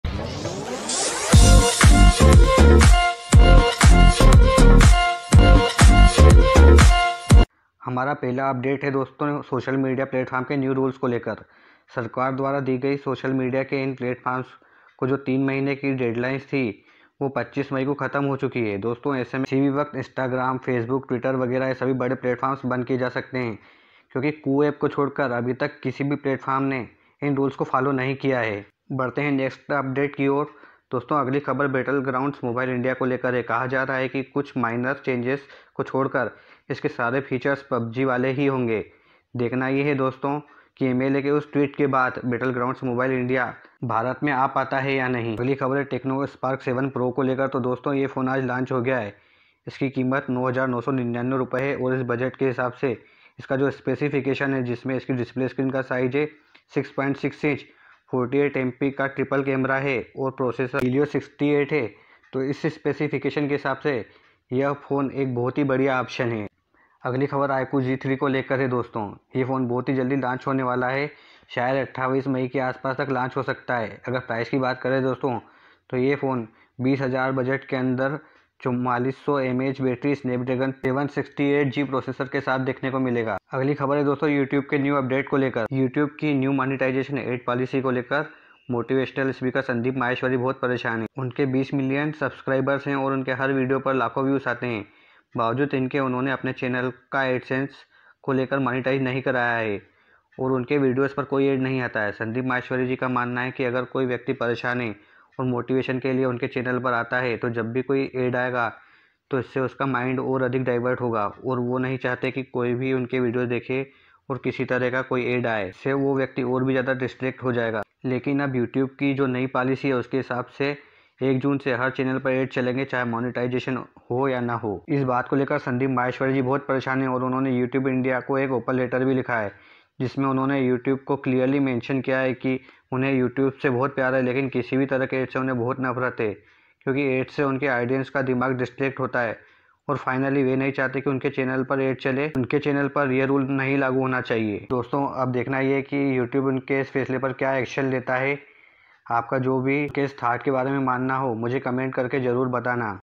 Twitter instagram ban | Pubg back again
Namaskar dosto, tech news and updates ki is episode me me laya hu apke liye ek se badkar ek dhamakedar updates so dont miss this episode. Mujhe ummid hai apko ye video bahot pasand aayegi
Topics covered in this video :
00:10 Facebook whatsapp twitter ban
00:28 Battlegrounds mobile india vs pubg
00:45 Techno spark 7 pro
01:06 Iqoo z3
01:25 Youtube monitization polici vs sandeep maheshwari
-Ravi Kumar